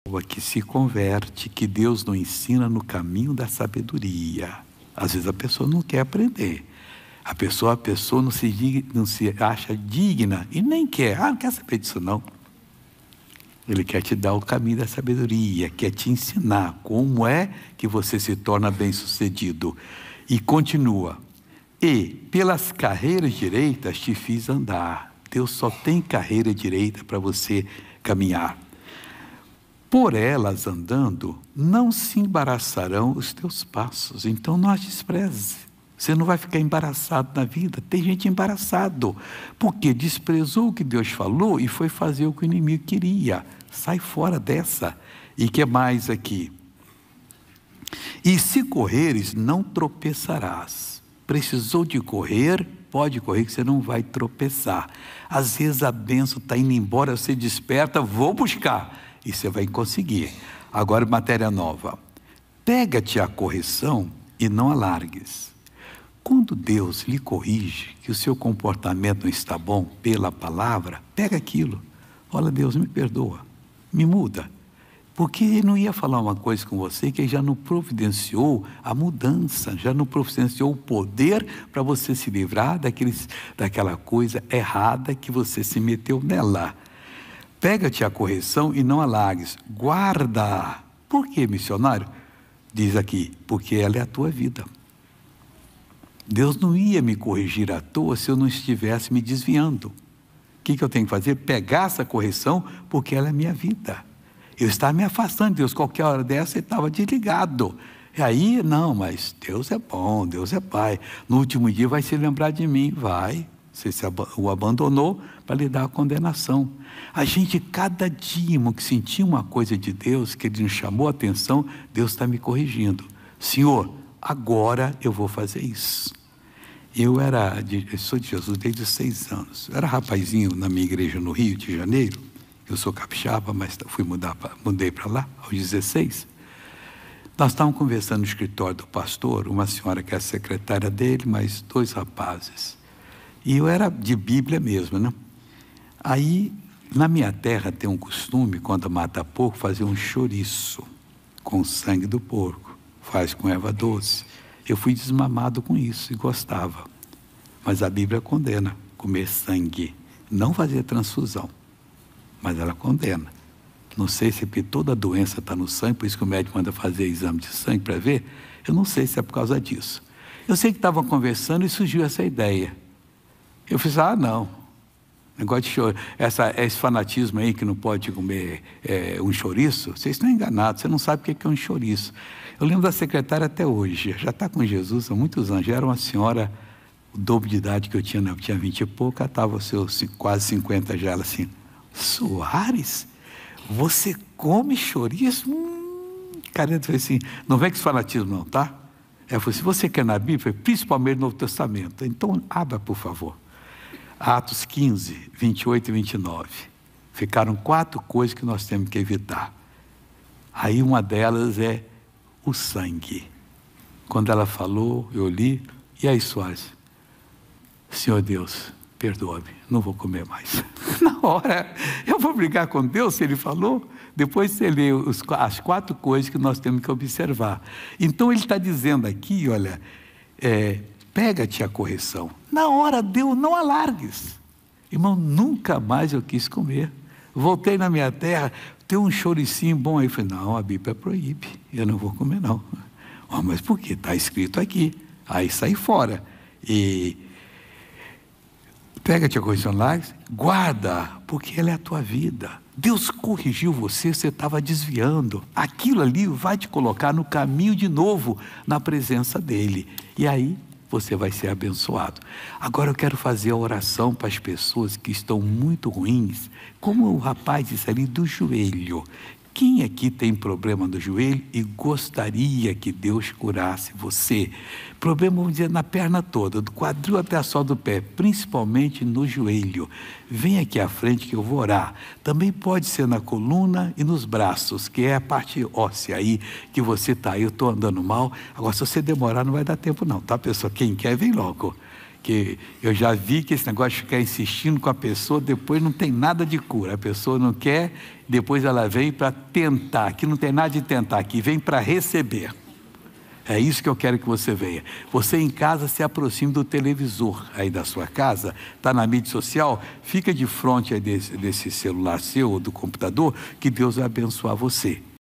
COMO APRENDER DA PALAVRA DE DEUS - MISSIONÁRIO RR SOARES - VEJA AGORA ESSA GRANDE MENSAGEM
Missionário RR Soares da igreja internacional da graça de Deus vem abençoando vidas através de seu ministério.

00:00 Início
00:23 Provérbios 4:11
05:00 Conclusão